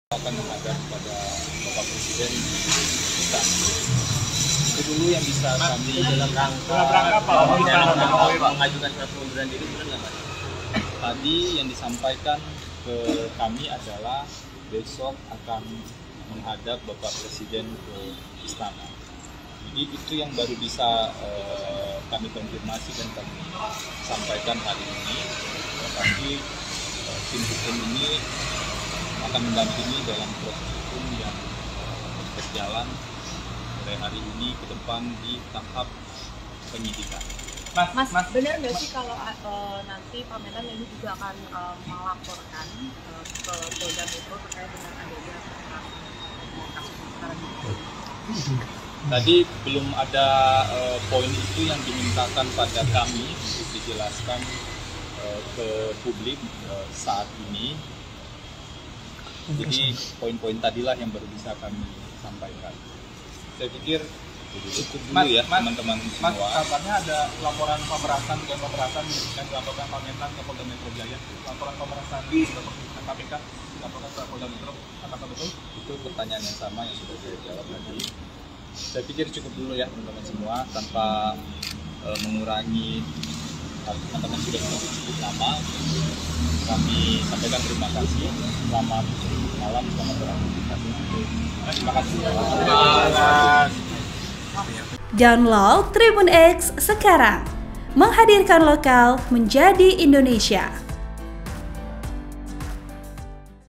Mentan Syahrul Yasin Limpo Menghadap Presiden Jokowi di Istana Hari Ini seusai 'Hilang' di Eropa
TRIBUN-VIDEO.COM - Syahrul Yasin Limpo sudah tiba di Indonesia setelah dikabarkan menghilang di Eropa di tengah pusaran dugaan kasus korupsi Kementan.

Setelah tiba di Indonesia pada Rabu (4/10/2023), Syahrul Yasin Limpo akan menghadap  Presiden Jokowi di Istana.

Pertemuan itu akan dilakukan pada hari ini, Kamis (5/10/2023).

Hal itu disampaikan oleh Mantan Jubir KPK, Febri Diansyah.

VP: Indra